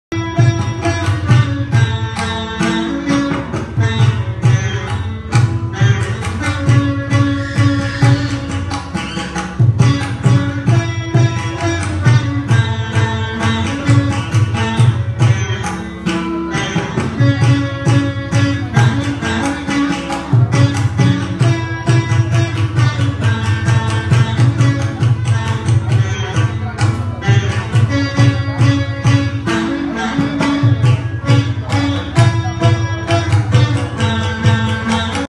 Veena Recital by Students of Sangeetha Pooshanam Ms. Indra Kumarasivam -Navarathiri Music Concert
Veena Recital by Students of Sangeetha Pooshanam Ms. Indra Kumarasivam
Veena Recital by Sangeetha Pooshanam   Ms. Indra Kumarasivam, Famous Veena Artist & Leading Veena Teacher in Sri Lanka and her Students, held during Navarathiri Music Concert at Mahasihnu Kovil, Dehiwella , Sri Lanka in 2017
Mridangam by A.G.S.Swaminathan sharma